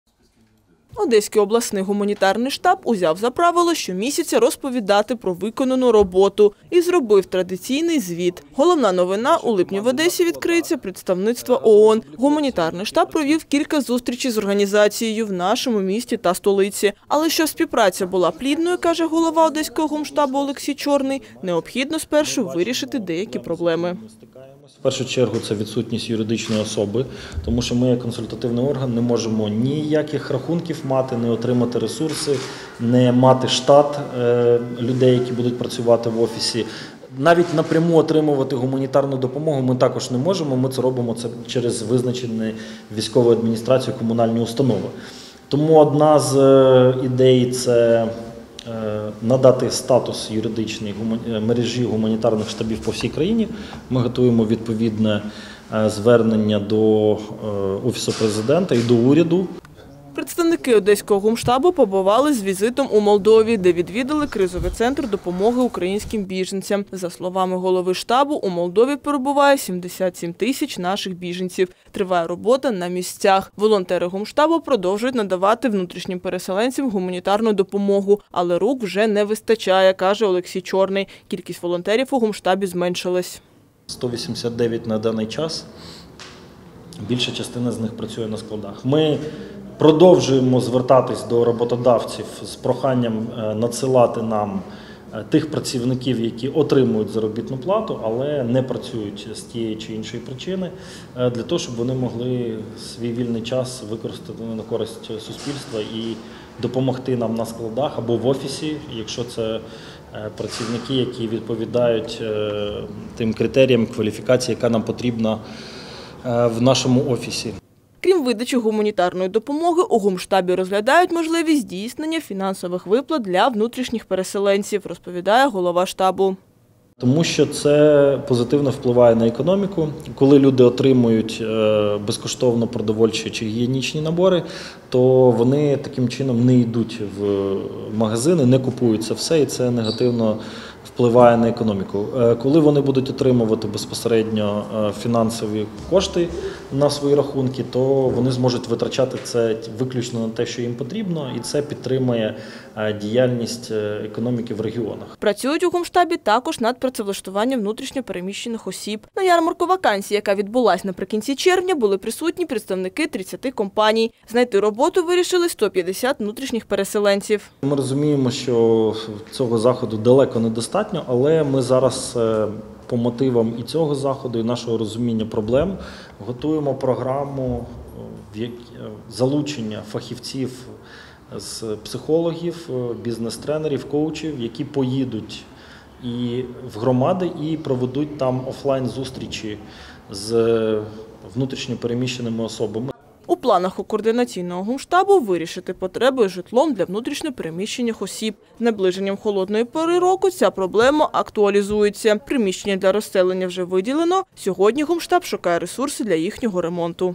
В Одесі відкриють представництво ООН
Представники Одесського гумштабу побували з візитом у Молдові, де відвідали кризовий центр допомоги українським біженцям. За словами голови штабу, у Молдові перебуває 77 тисяч наших біженців. Триває робота на місцях. Волонтери гумштабу продовжують надавати внутрішнім переселенцям гуманітарну допомогу. Але рук уже не вистачає – каже Олексій Чорний. Кількість волонтерів у гумштабі зменшилась.
Крім видачі гуманітарної допомоги, у гумштабі розглядають можливість здійснення фінансових виплат для внутрішніх переселенців, розповідає голова штабу.
Працюють у гумштабі та над працевлаштуванням внутрішньо переміщених осіб. На ярмарку вакансій, яка відбулась наприкінці червня, були присутні представники 30-ти компаній. Знайти роботу вирішили 150 внутрішніх переселенців.
У планах у координаційного гумштабу - вирішити потреби з житлом для впо. З наближенням холодної пори року ця проблема актуалізується. Приміщення для розселення вже виділено, сьогодні гумштаб шукає ресурси для їхнього ремонту.